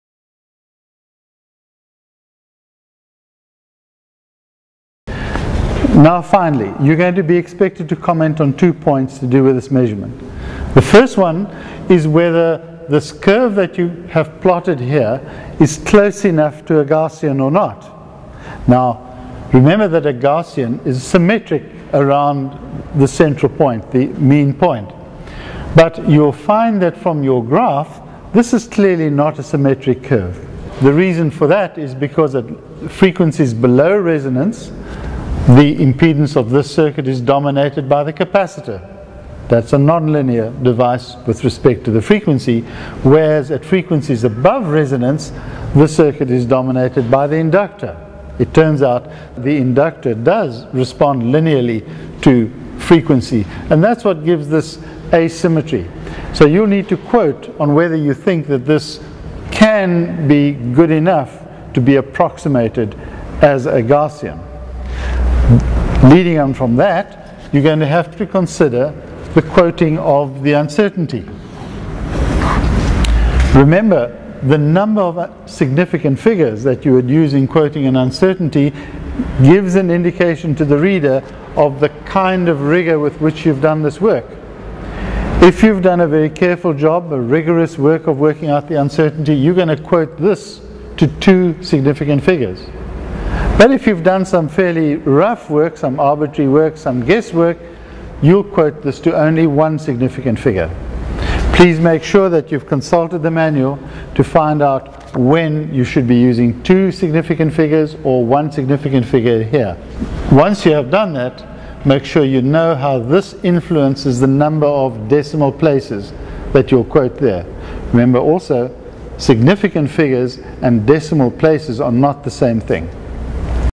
2 5 3 LRC comment and quote R1
Describes the requirement for the student to comment on the problem of using a normal distribution in their analysis when the distribution of the data are not normal.